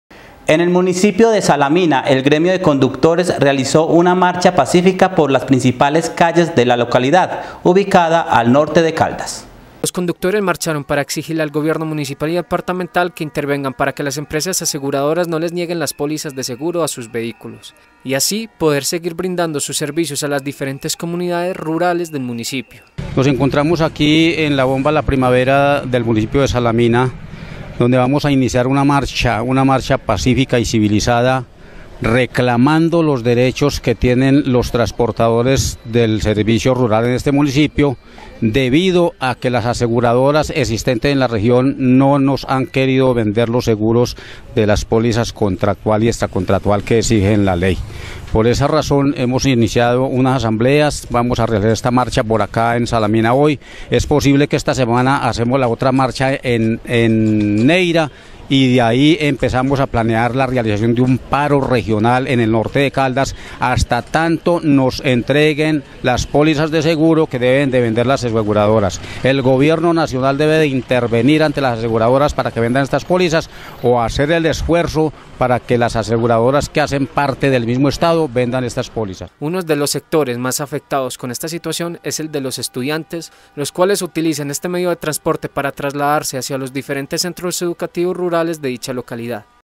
MARCHA DE CONDUCTORES EN SALAMINA
En el municipio de Salamina el gremio de conductores realizó una marcha pacífica por las principales calles de la localidad, ubicada al norte del departamento de Caldas.